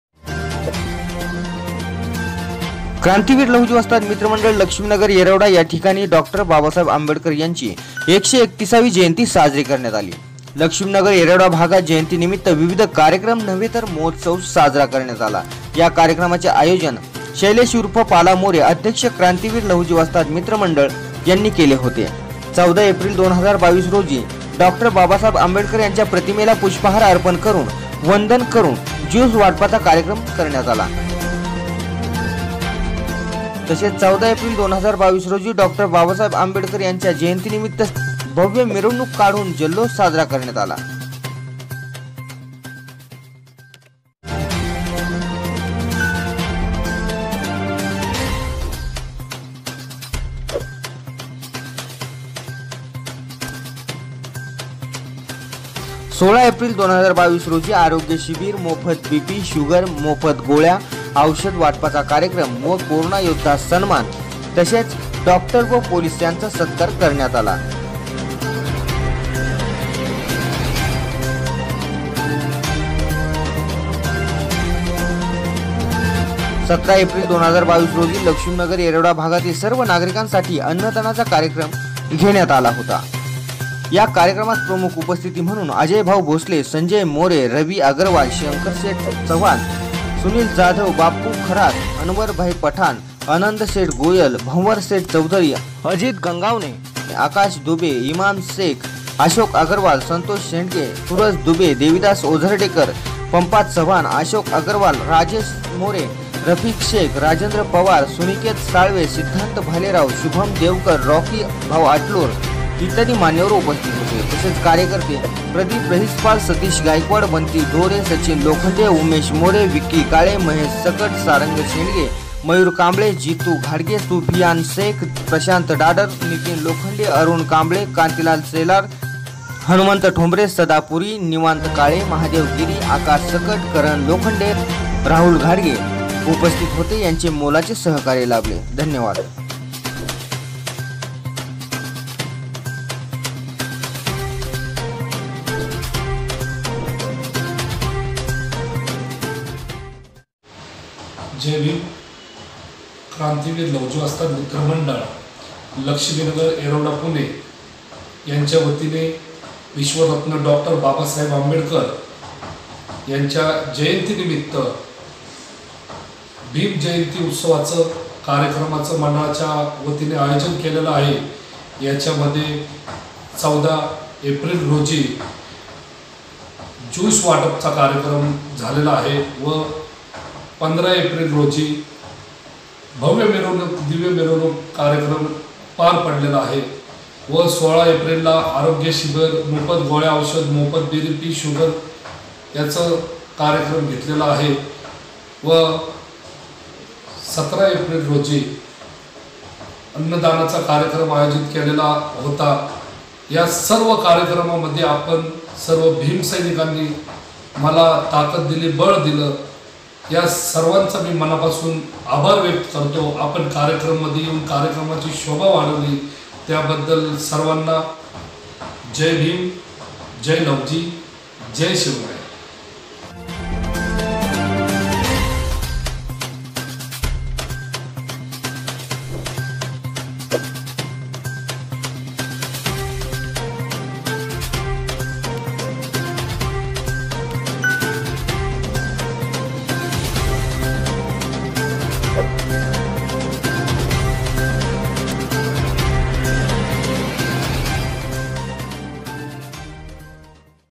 क्रांतिवीर लहुजी वस्ताद मित्र मंडळ येरवडा यांच्यावतीने डॉ बाबासाहेब आंबेडकर यांची १३१वी जयंती साजरी
क्रांतिवीर लहुजी वस्ताद मित्र मंडळ येरवडा यांच्या वतीने डॉ बाबासाहेब आंबेडकर यांची १३१ वी जयंती साजरी